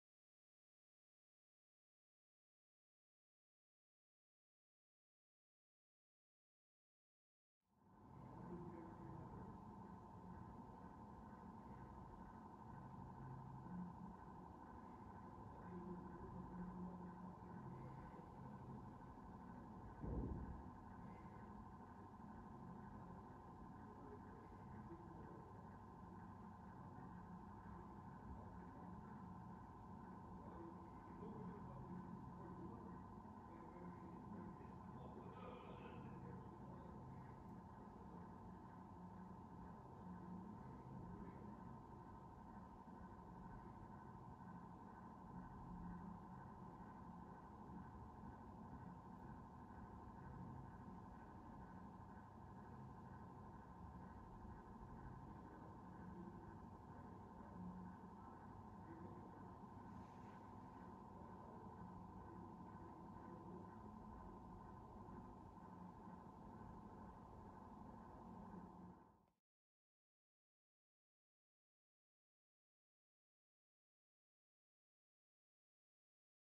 Schrei hinter Gitter
Johannes M. L. Pasquay montiert Pochier-Schablonen nach Gebrauch zu einem neuen Objekt.